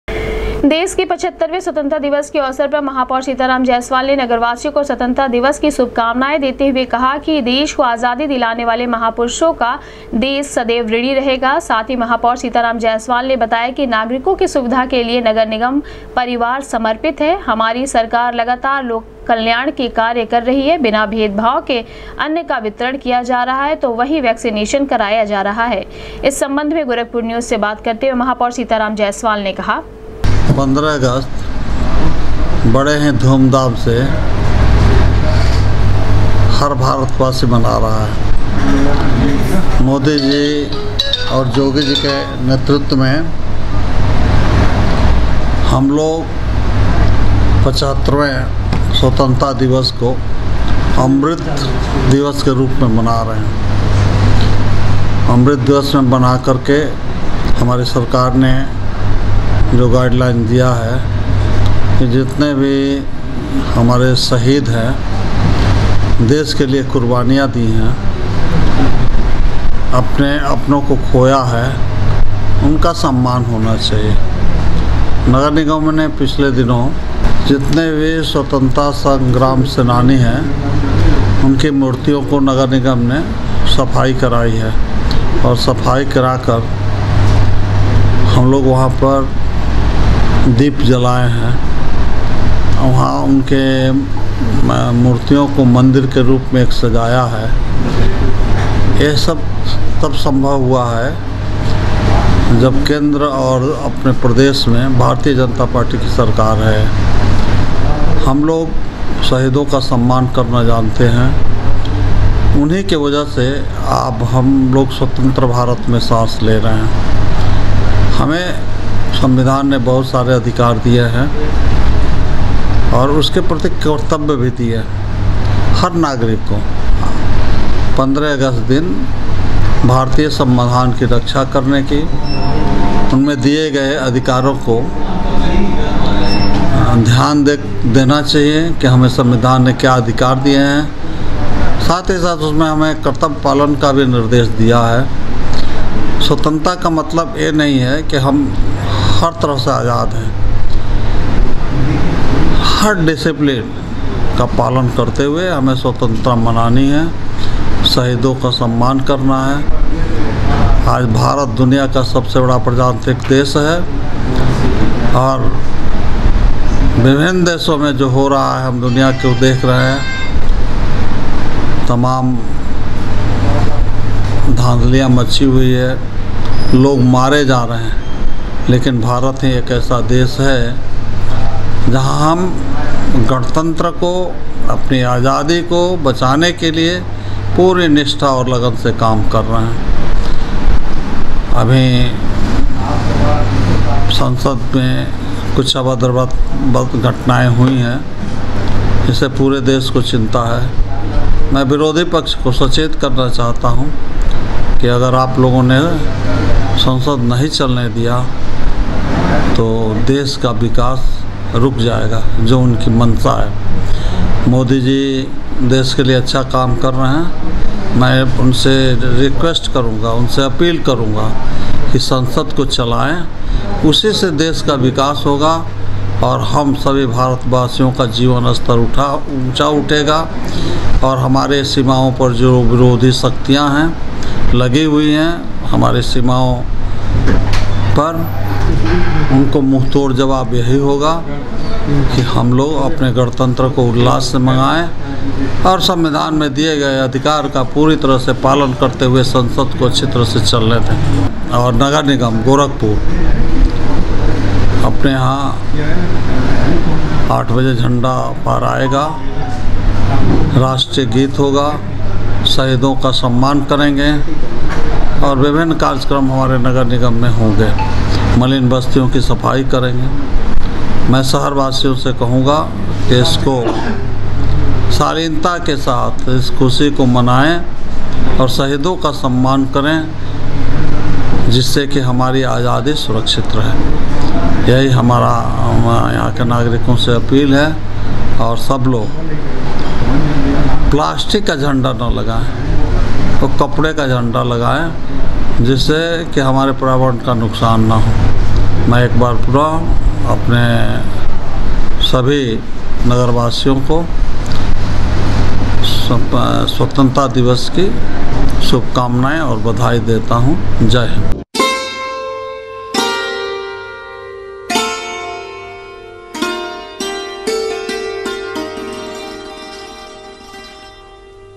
75 वें स्वतंत्रता दिवस के अवसर पर महापौर सीताराम जायसवाल ने नगरवासियों को दी शुभकामनाये, कहा, नगर
75 वें स्वतंत्रता दिवस के अवसर पर महापौर सीताराम जायसवाल ने नगरवासियों को दी शुभकामनाये, कहा, नगर निगम परिवार नागरिकों के सुविधाओं के लिए है समर्पित, बिना भेदभाव के साथ निगम कर रही है काम